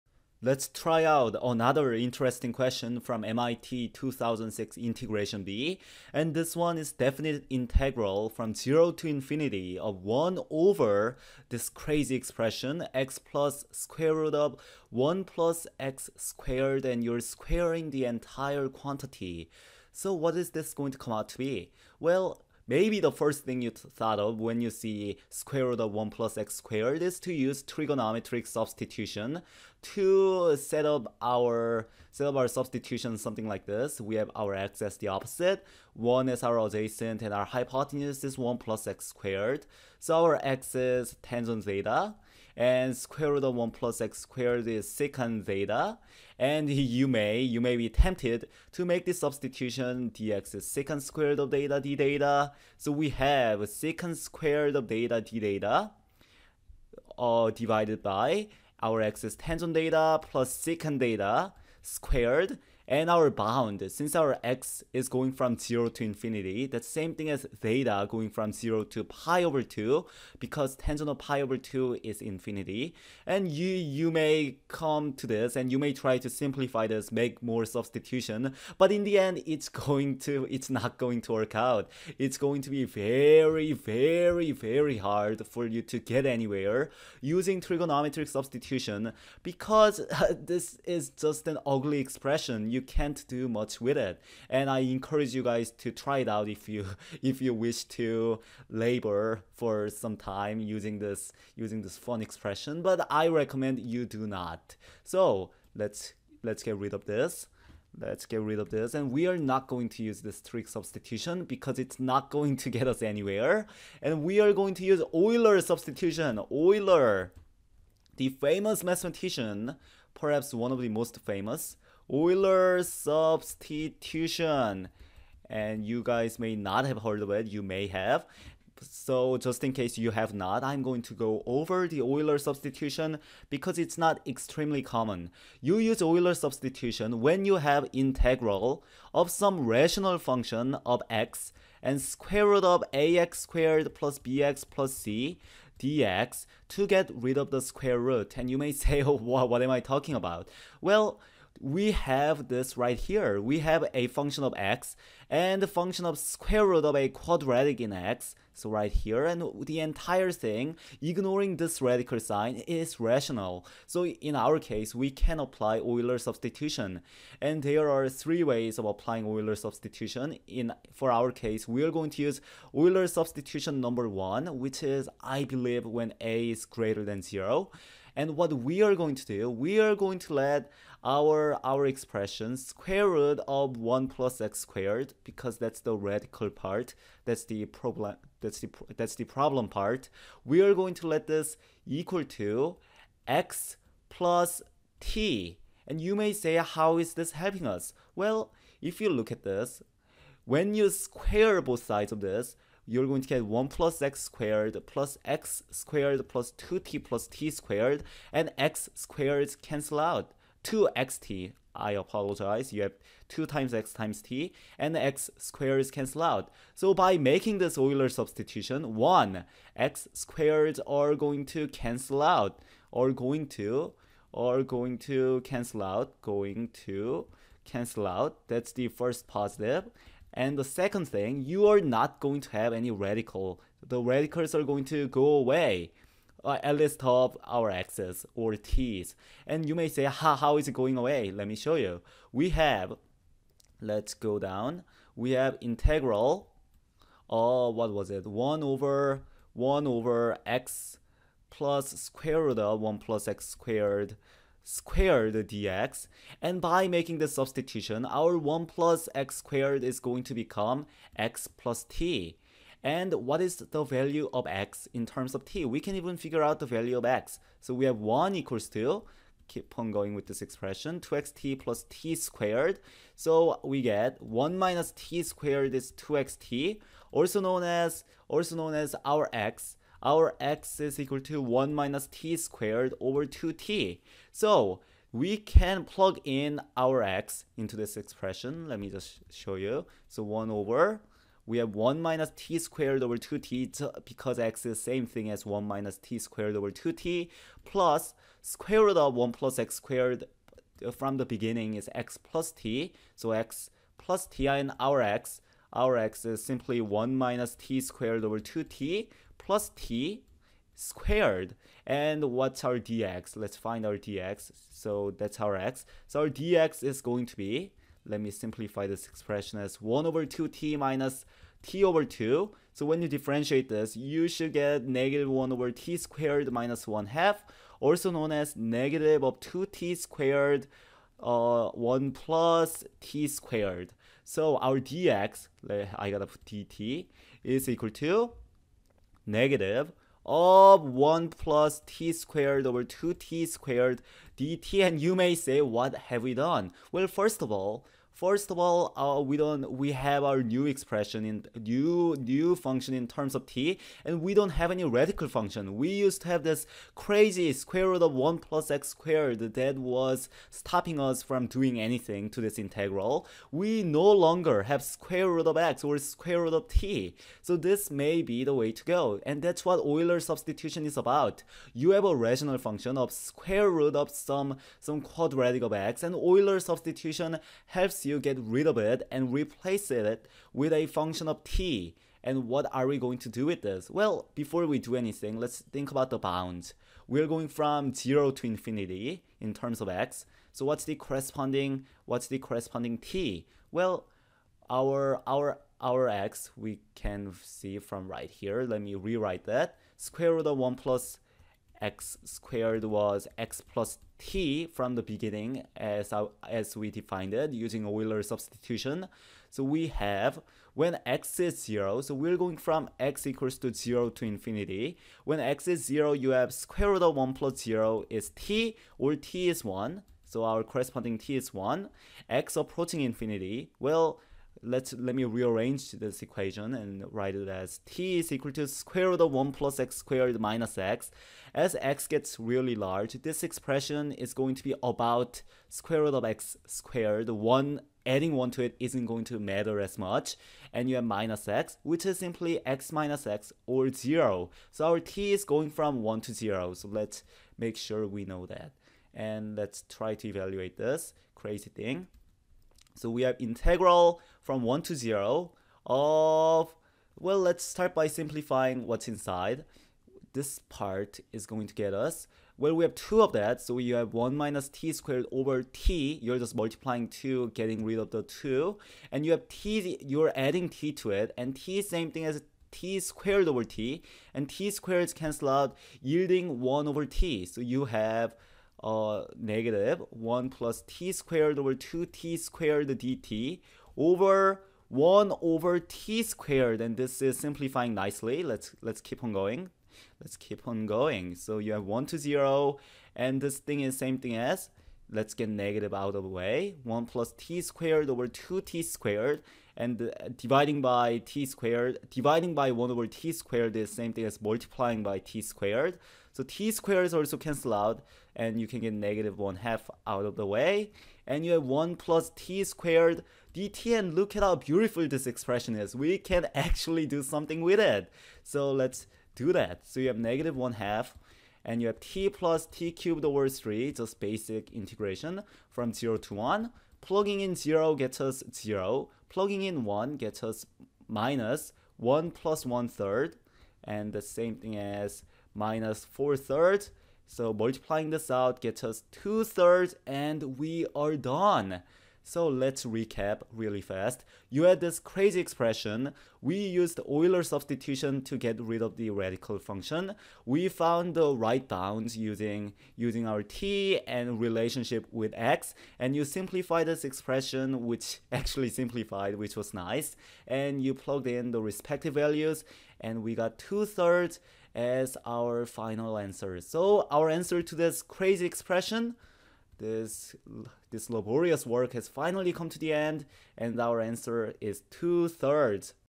Euler Substitution: MIT Integration Bee (3)
Let's use Euler Substitution to evaluate integral from 0 to infinity of 1/(x+sqrt(1+x^2))^2.